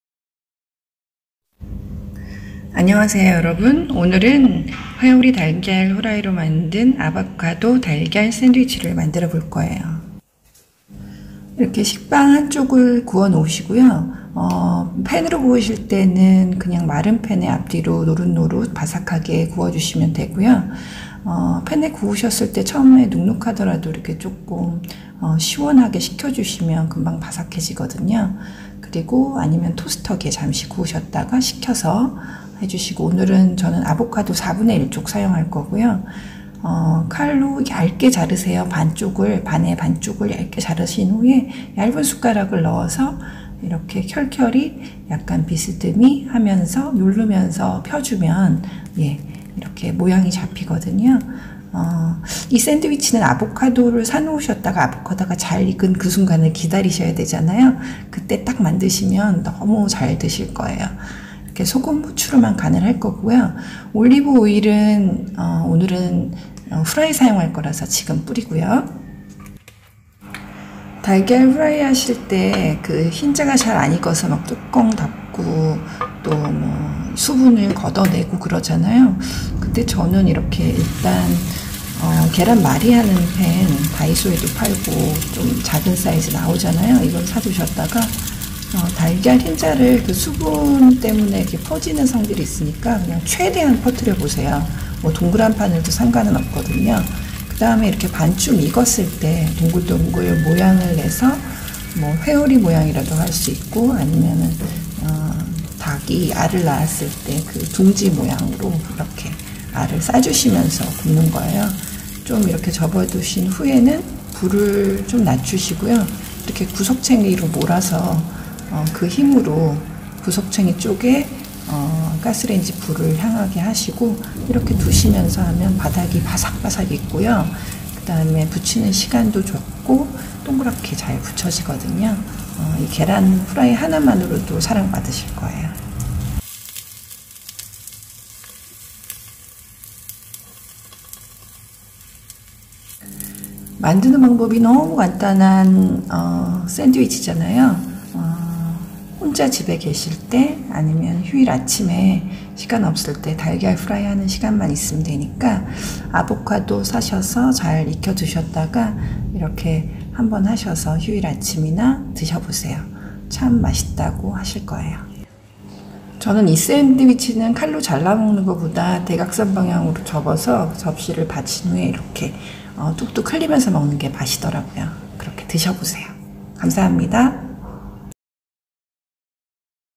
계란후라이 예쁘게 하는법 [Beautiful Sunny Side-up Egg] 🥚 🍳
계란후라이 잘하는법, 아보카도 손질하는법, 아보카도 샌드위치 만들기
How to make Perfect Sunny  Side up egg
How to eat Avocado